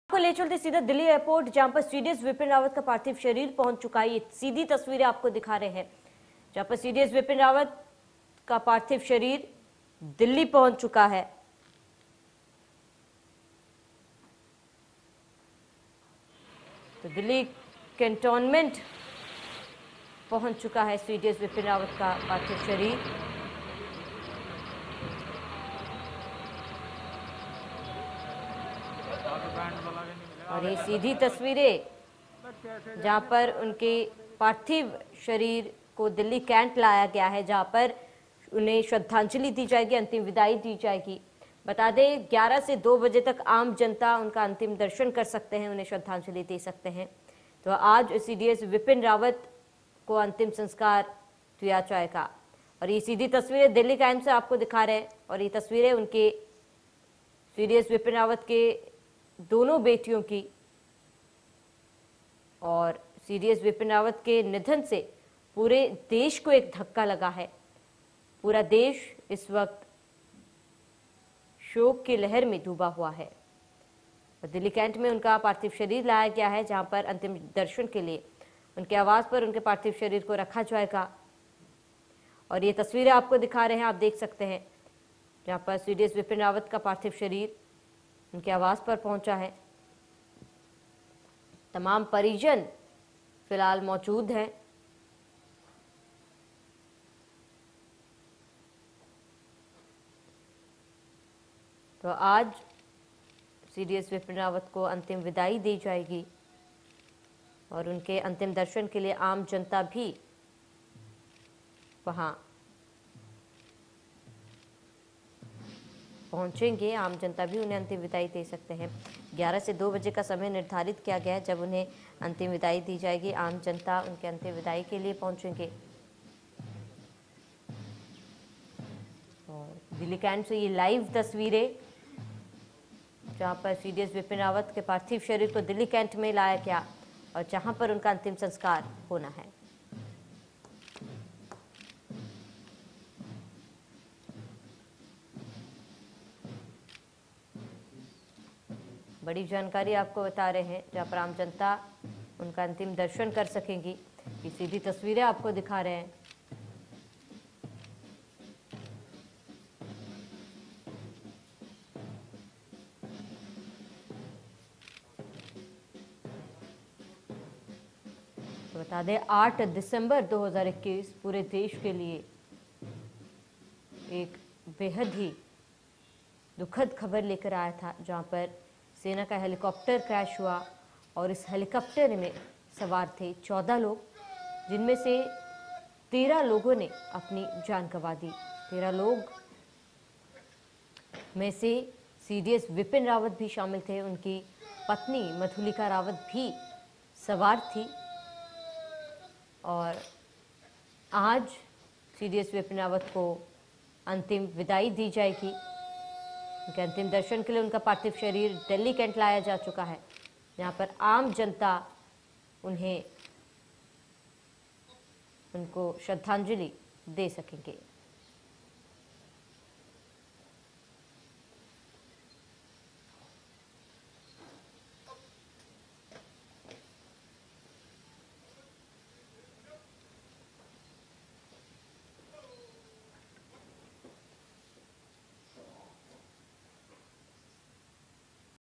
Delhi : CDS रावत के आवास पर पार्थिव शरीर रखा गया, आम जनता कर सकेगी अंतिम दर्शन
Delhi : CDS रावत के आवास पर पार्थिव शरीर रखा गया, आम जनता कर सकेगी अंतिम दर्शन | India News UPUK